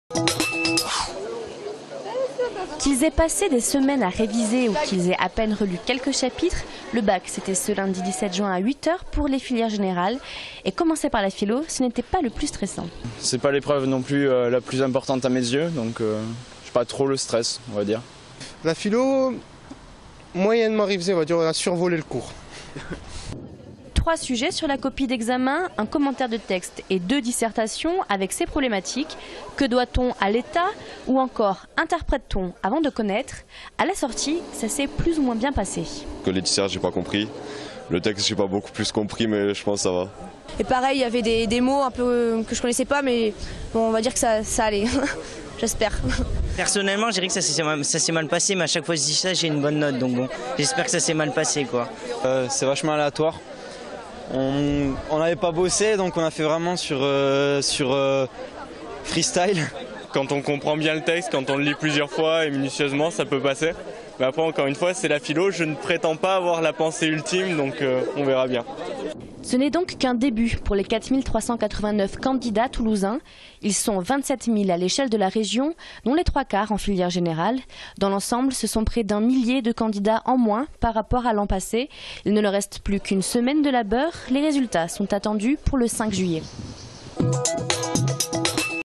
Début des épreuves du Bac (Toulouse)
Sites webs/blogs, copiez cette vidéo avec le bouton "Intégrer" !
 Les impressions de ceux qui ont commencé les épreuves du bac par l'épreuve de philosophie.